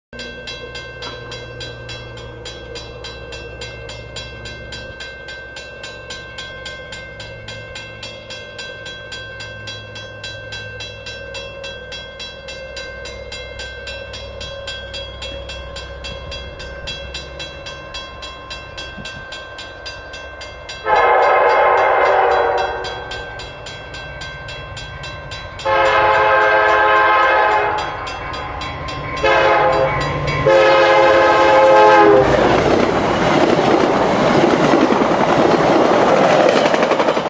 VIA 49 at Bellamy on the Brockville Sub
Passing through Bellamy on its way to Brockville and then on to Toronto